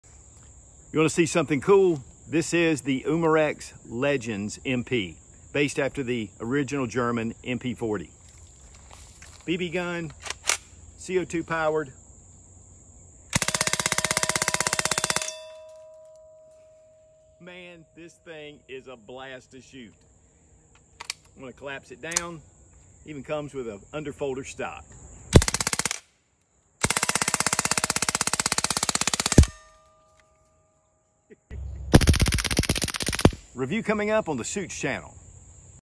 MP-40 BB SMG! Umarex Legends MP
Umarex Legends MP is a faithful replica of the MP-40 using C02 and BBs. It’s a blast